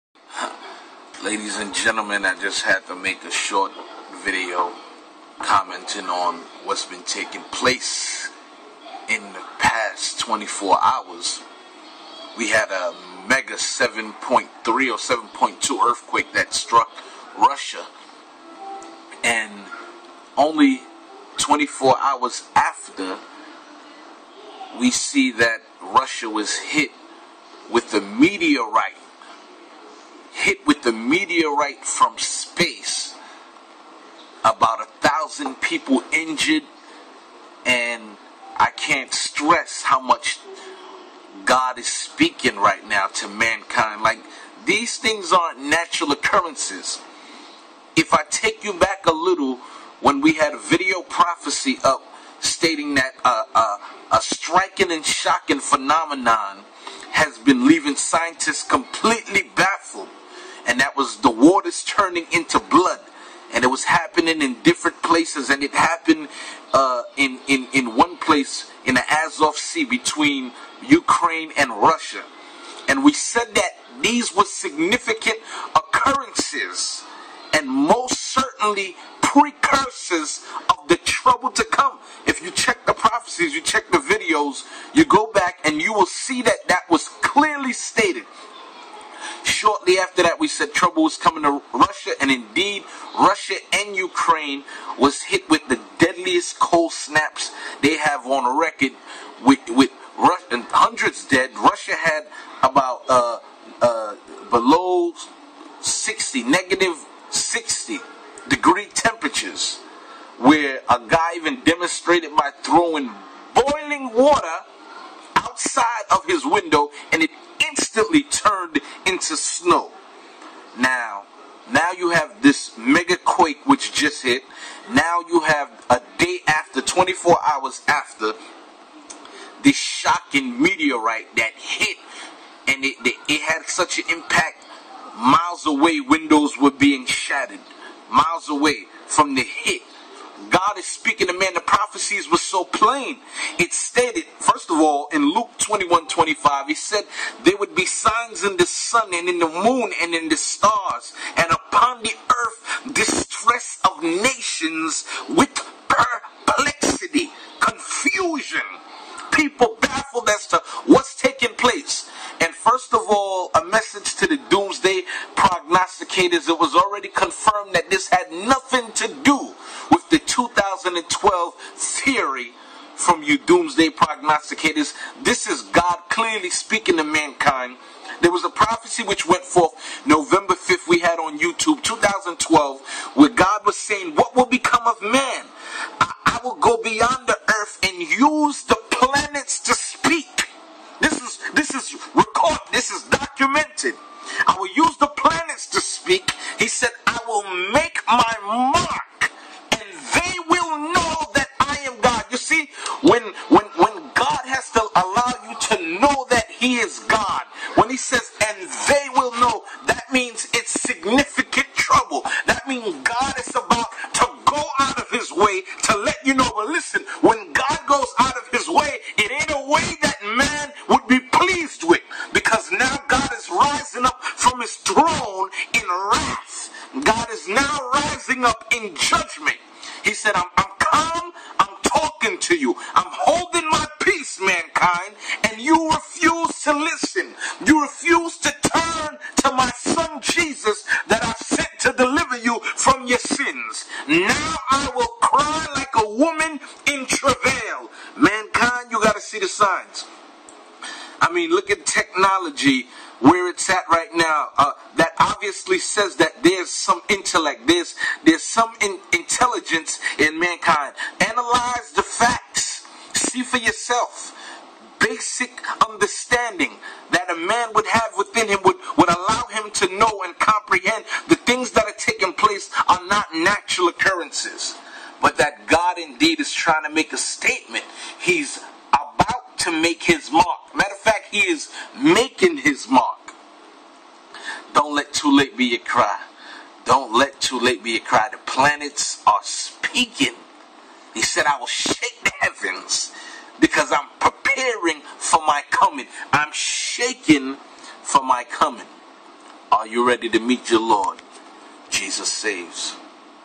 RUSSIA - METEOR SPACEQUAKE & 7.3 MEGAQUAKE - Feb.15,2013 'Broadcoast Engineer' Unhinged
[must see]
Feb.15,2013 Harvest Army Senior Broadcast Engineer speaks out spontaneously, shoecked by the apocalyptic megaquakes and meteorite spacequakes ravaging the earth.

Feb.15,2013 A very destructive meteor explosion has rocked central Russia only 24 hours after a 7.3 Megaquake struck the country The meteorite explosion caused much damage, injury and panic. Some scientists believe that it is connected to the asteroid due to pass the earth 17,000 away

- Central Rusia
- Almost 1,000 Injured
- Fireballs Crash to the earth
- Buildings Damaged
- Windows Shattered
- Long white Trail could be seen for 125 mls
- Thinderous Explosive Sound
- Vehicle Alarms went off
- Travelled 19 mls/hr
- A blinding effect
- Might be linked to an asteroid due to pass earth 17,100 mls away
- Never like this in Russia before, a meteor breaking uo at such low level
- Shockwaves felt in many building afar off

HOW TO BE SAVED:
To The Honest Enquirer, PRAY the following prayer or one similar: